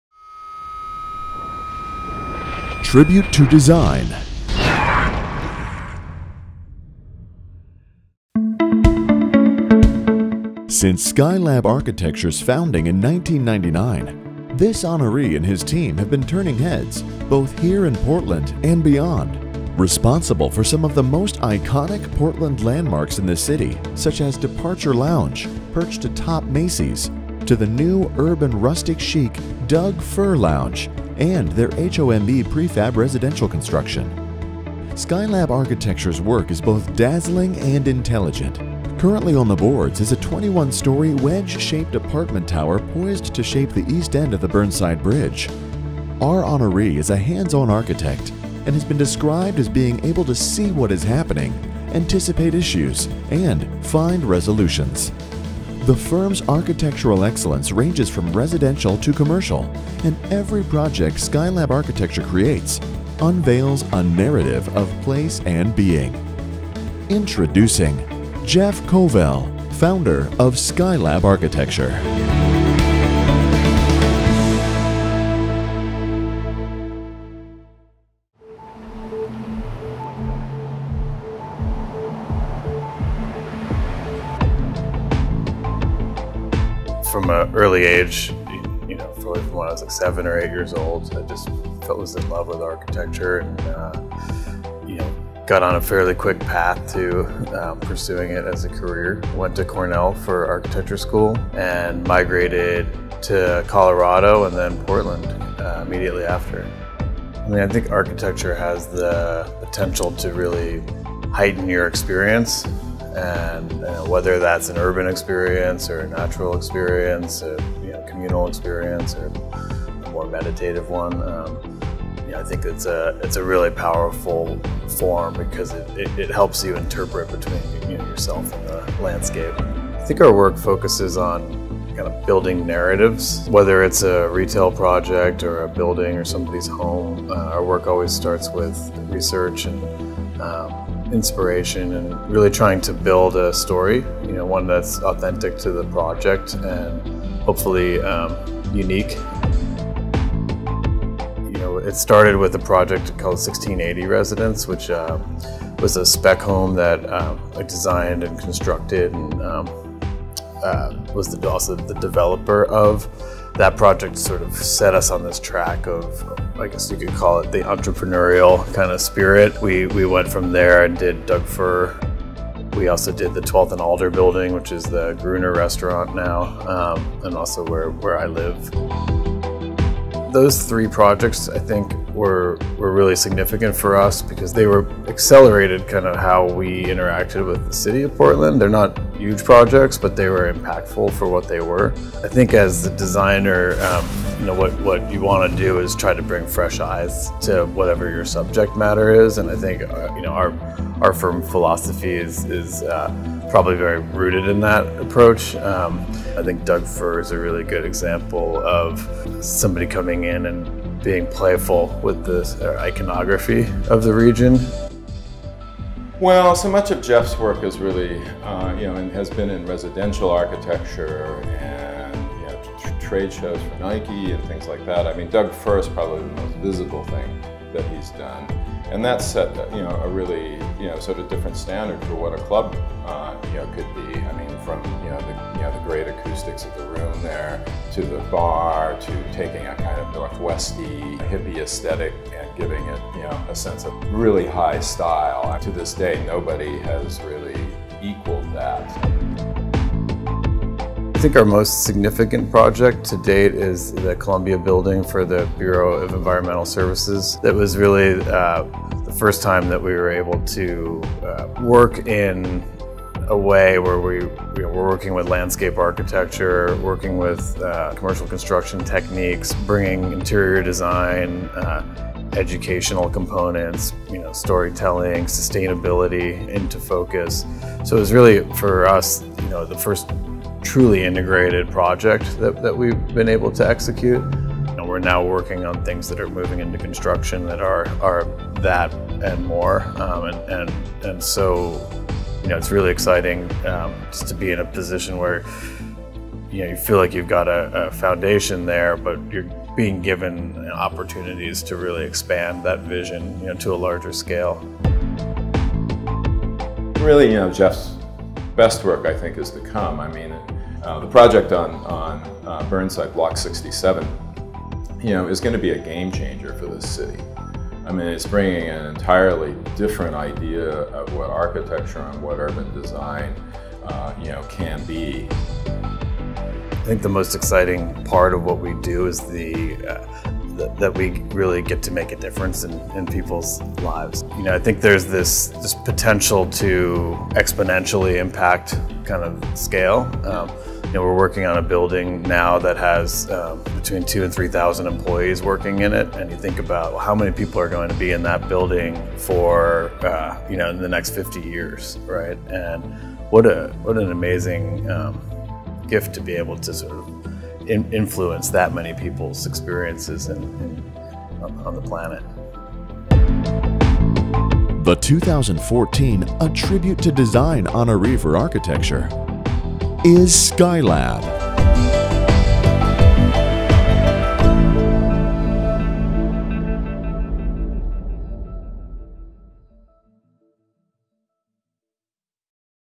A Tribute to Design - Skylab Architecture
Since Skylab Architecture’s founding in 1999, this honoree and his team have been turning heads both here in Portland and beyond. Responsible for some of the most iconic Portland landmarks in the city such as Departure Lounge perched atop Macy’s to the new-urban rustic chic Doug Fir Lounge and their HOMB prefab residential construction.  Skylab Architecture’s work is both dazzling and intelligent. Currently on the boards is a 21 story, wedge shaped apartment tower poised to shape the East End of the Burnside Bridge. Our Honoree is a hands-on architect and has been described as being able to see what is happening, anticipate issues and find resolutions. The firm’s architectural excellence ranges from residential to commercial, and every project Skylab Architecture creates… unveils a narrative of place and being.